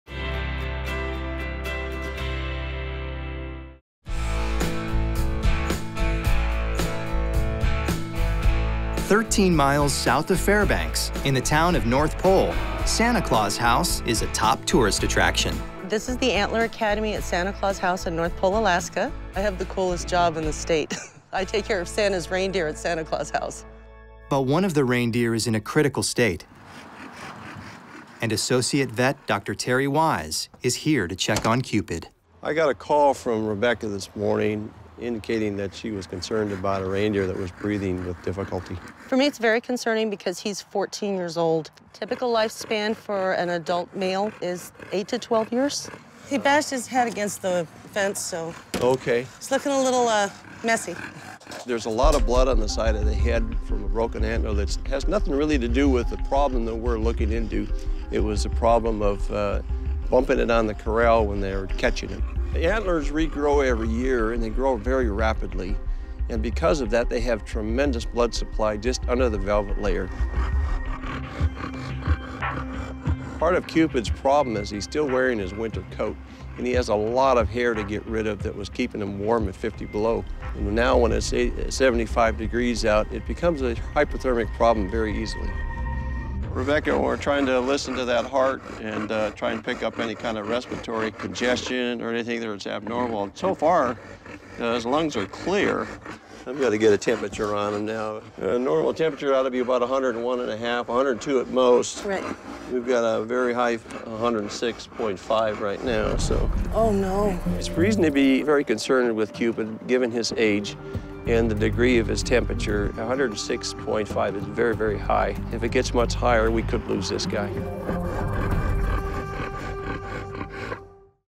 Reindeer Cupid Is Under the Weather
Dr. Terry heads to Santa Claus House in North Pole, Alaska, to check on Cupid, a reindeer that appears to be having breathing issues and a broken antler.